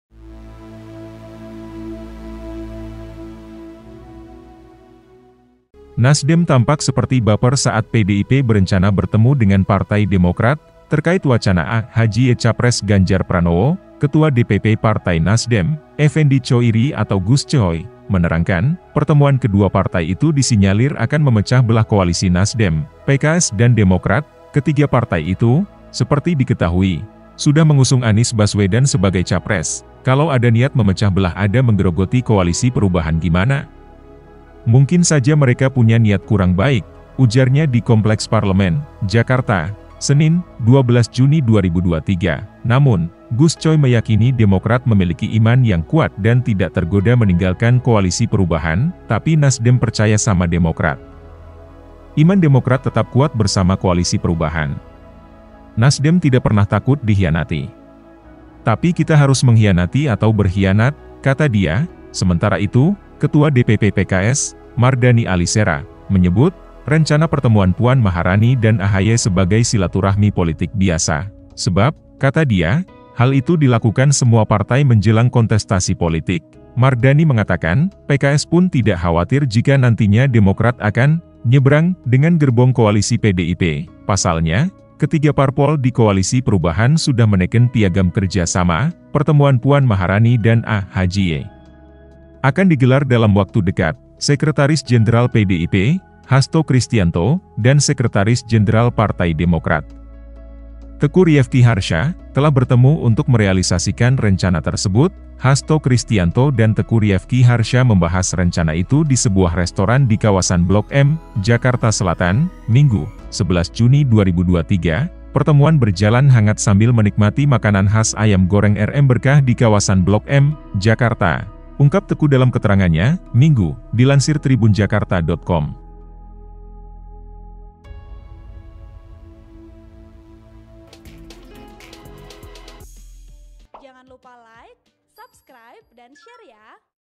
BAPERNYA NASDEM SAAT PDIP-DEMOKRAT MAU KETEMU, BAHAS AHY CALON CAWAPRES GANJAR PRANOWO
TRIBUNJABARVIDEO- Nasdem tampak seperti baper saat PDIP berencana bertemu dengan Partai Demokrat, terkait wacana AHY capres Ganjar Pranowo.

Ketua DPP Partai NasDem, Effendi Choiri atau Gus Choi, menerangkan, pertemuan kedua partai itu disinyalir akan memecah belah koalisi Nasdem, PKS dan Demokrat.

Ketiga partai itu, seperti diketahui, sudah mengusung Anies Baswedan sebagai capres.

"Kalau ada niat memecah belah ada menggerogoti Koalisi Perubahan gimana? Mungkin saja mereka punya niat kurang baik," ujarnya di Kompleks Parlemen, Jakarta, Senin (12/6/2023).

Namun, Gus Choi meyakini Demokrat memiliki iman yang kuat dan tidak tergoda meninggalkan Koalisi Perubahan.

"Tapi NasDem percaya sama Demokrat. Iman Demokrat tetap kuat bersama koalisi perubahan. NasDem tidak pernah takut dikhianati. Tapi kita harus mengkhianati atau berkhianat," kata dia.

Sementara itu, Ketua DPP PKS, Mardani Ali Sera, menyebut, rencana pertemuan Puan Maharani dan AHY sebagai silaturahmi politik biasa.

Sebab, kata dia, hal itu dilakukan semua partai menjelang kontestasi politik.

Mardani mengatakan, PKS pun tidak khawatir jika nantinya Demokrat akan 'nyebrang' dengan gerbong koalisi PDIP.

Pasalnya, ketiga parpol di Koalisi Perubahan sudah meneken piagam kerja sama.

Pertemuan Puan Maharani dan AHY akan digelar dalam waktu dekat.

Sekretaris Jenderal PDIP, Hasto Kristiyanto, dan Sekretaris Jenderal Partai Demokrat, Teuku Riefky Harsya, telah bertemu untuk merealisasikan rencana tersebut.

Hasto Kristiyanto dan Teuku Riefky Harsya membahas rencana itu di sebuah restoran di kawasan Blok M, Jakarta Selatan, Minggu (11/6/2023).

"Pertemuan berjalan hangat sambil menikmati makanan khas Ayam Goreng RM Berkah di kawasan Blok M, Jakarta," ungkap Teuku dalam keterangannya, Minggu, dilansir TribunJakarta.com.(*)